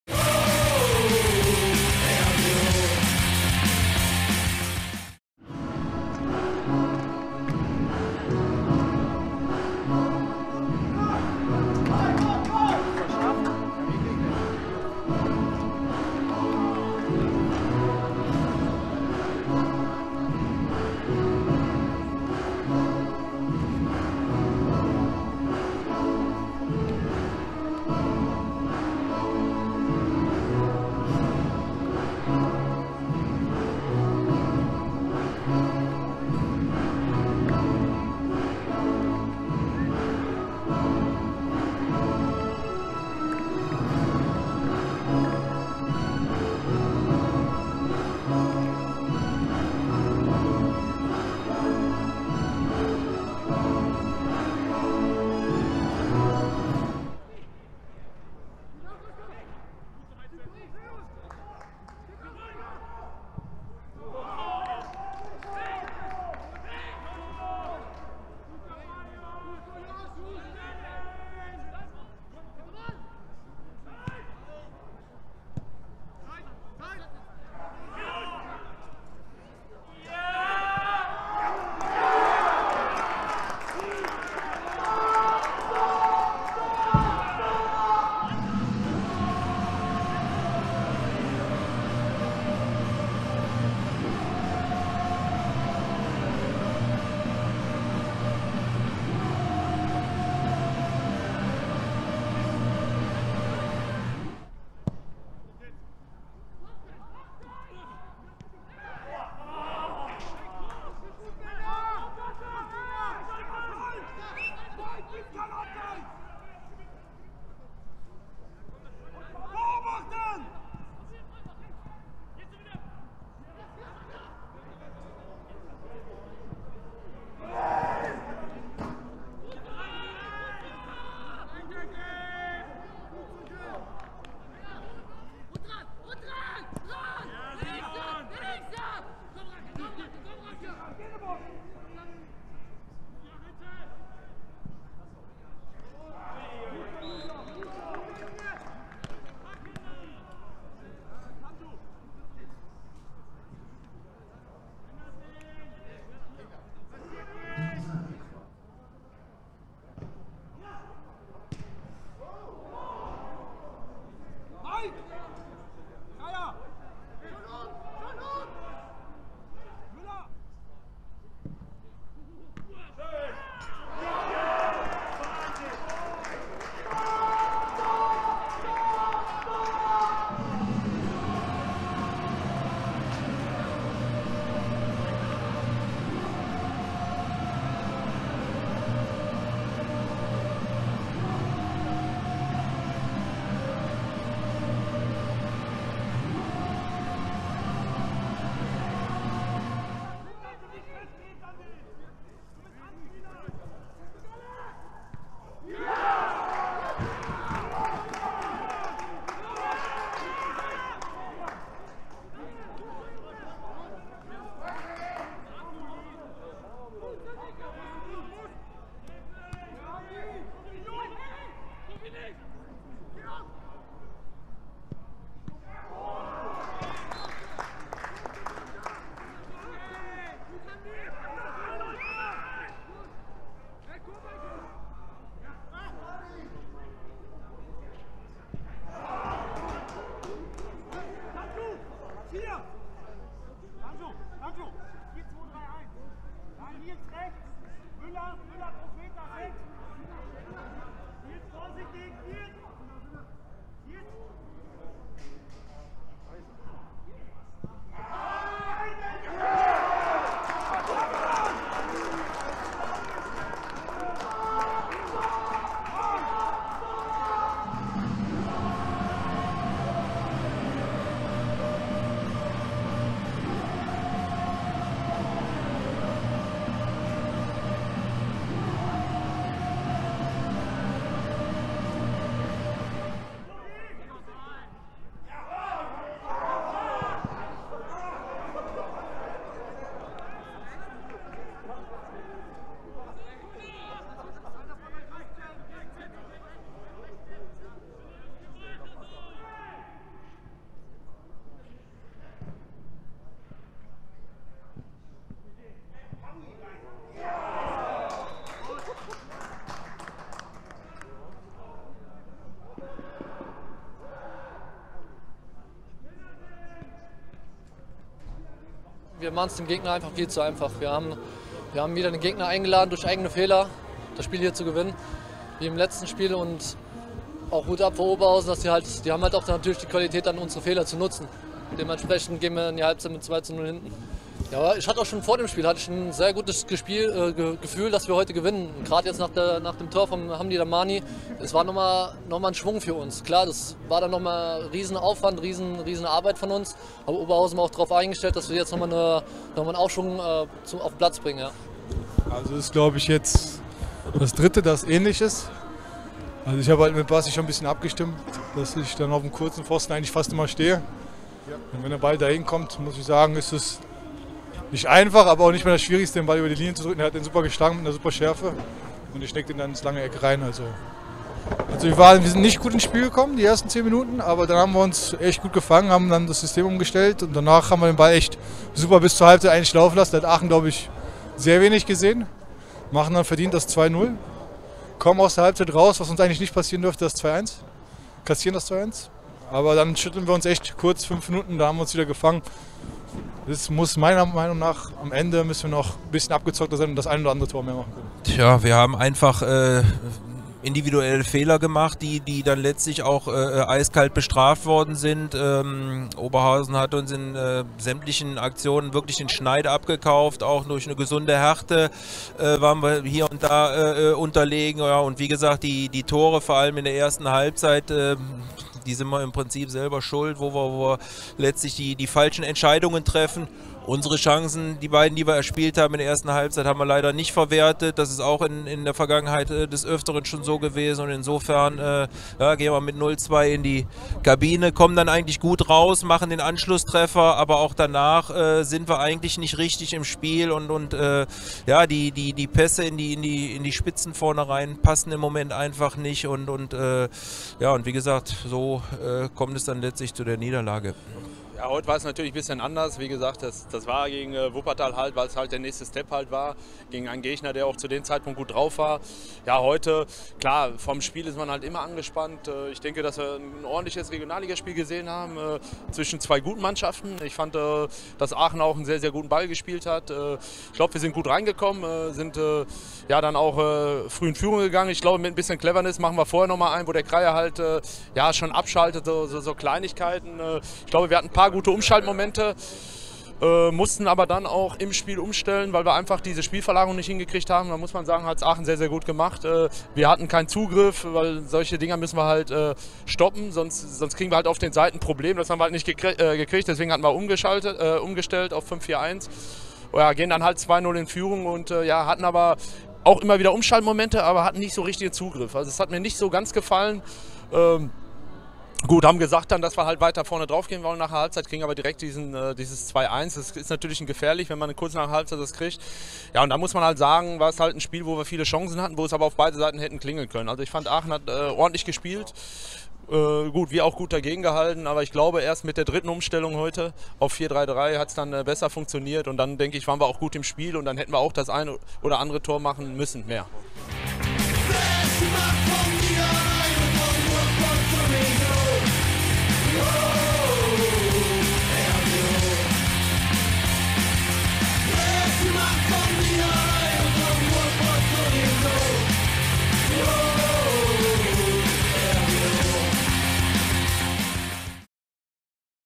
RWO.tv - Highlights: RWO gegen TSV Alemannia Aachen
Vier-Chancen-Tournee – dritter Teil: alle Highlights vom 3:1 Sieg gegen die Alemannia.